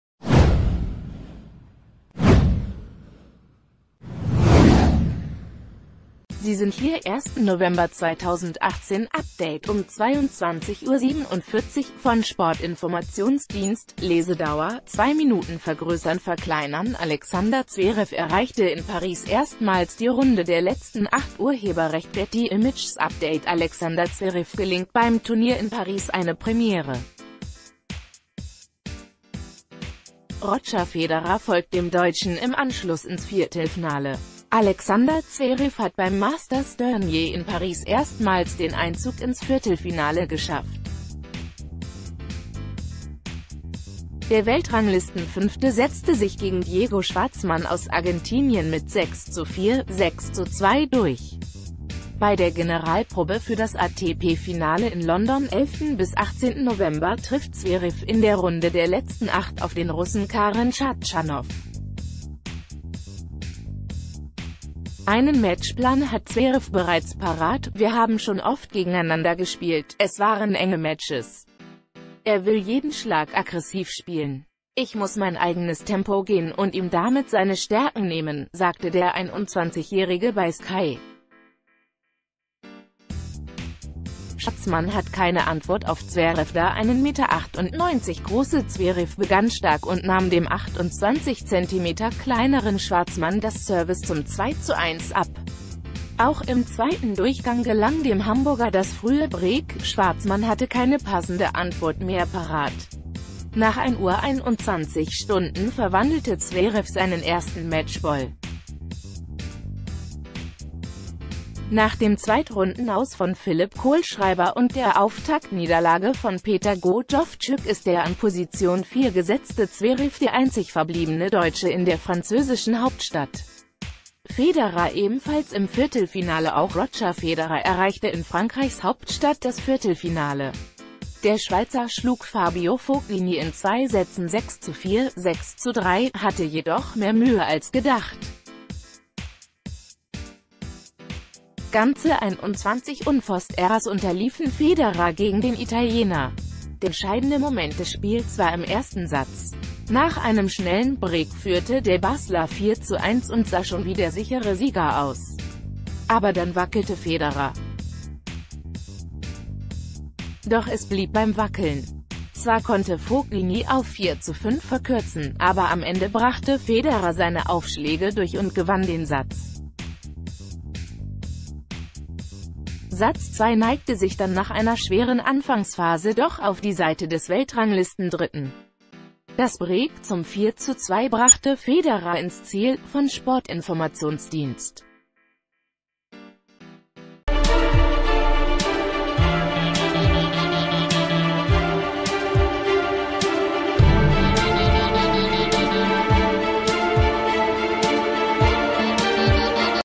Tennis: Alexander Zverev im Paris-Viertelfinale - auch Federer weiter
Sie sind hier: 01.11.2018 / Update um 22:47 Uhr - von Sportinformationsdienst / Lesedauer: 2 Minuten vergrößernverkleinern Alexander Zverev erreichte in Paris erstmals die Runde der letzten Acht © Getty ImagesUpdate Alexander Zverev gelingt beim Turnier in Paris eine Premiere. Roger Federer folgt dem Deutschen im Anschluss ins Viertelfnale. Alexander Zverev hat beim Masters-Turnier in Paris erstmals den Einzug ins Viertelfinale geschafft. Der Weltranglistenfünfte setzte sich gegen Diego Schwartz...